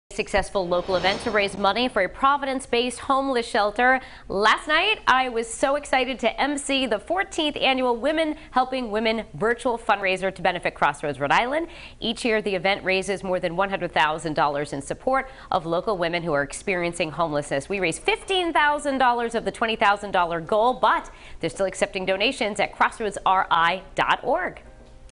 Women Helping Women virtual fundraiser raises money for women experiencing homelessness
Crossroads Rhode Island's 14th Annual Women Helping Women fundraiser was once again held virtually this year.